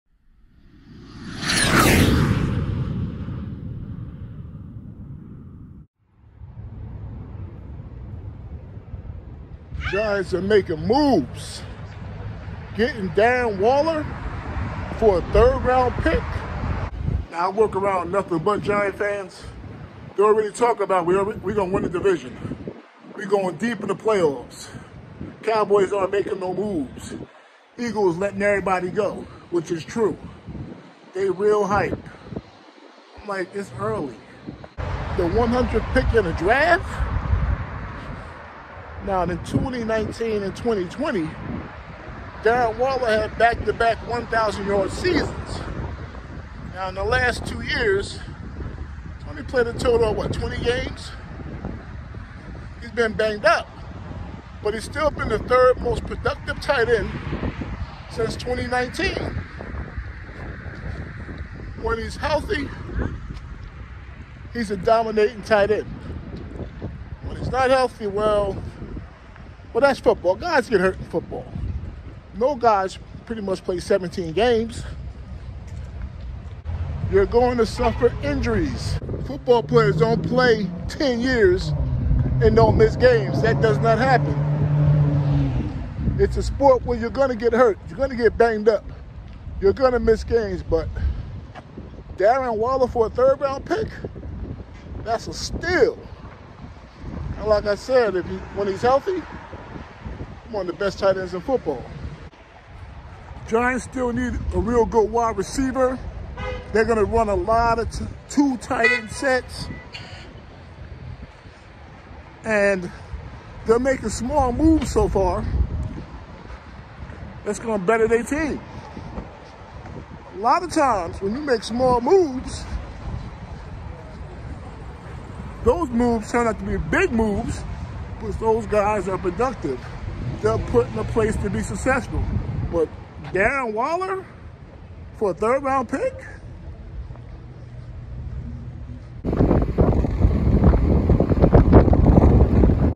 NY Giants Get Darren Waller for A 3rd Round Pick THATS A STEAL
The Giants will send a third-round draft pick (No. 100 overall) to the Raiders for Waller, a 2020 Pro Bowler who will provide quarterback Daniel Jones with an established veteran target in the passing game.

Waller had back-to-back seasons of over 1,000 receiving yards in 2019 and 2020 but has been plagued by multiple injuries over the past two seasons, appearing in just 20 total games over that span. Waller still ranks third among NFL tight ends in catches (280) and receiving yards (3,394) since 2019, behind Travis Kelce and Mark Andrews.

The draft pick headed to Las Vegas was originally acquired by the Giants from the Chiefs this past October in the trade that sent receiver Kadarius Toney to Kansas City.